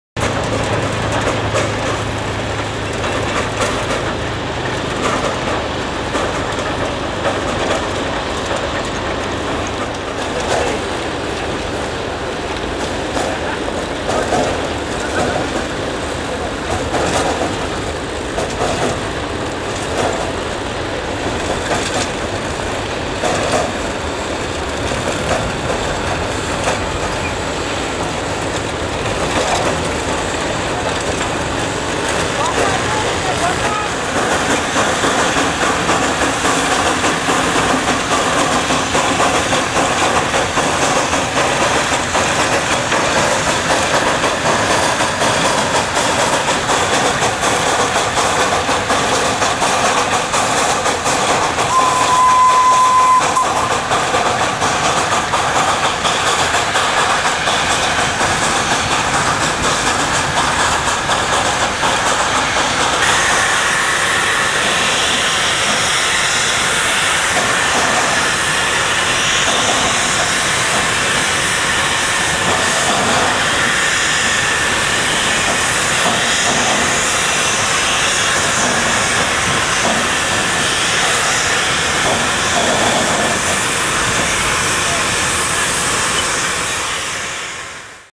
Dolok Ilir Palm Oil Mill, North Sumatra, Indonesia, Part 1
This is another in a series of historic clips shot on Video 8 (hence the picture quality) between 1991 and 1999. Several 600mm gauge Maffei 0-6-0T at work within the mill complex at Dolok Ilir in August 1991.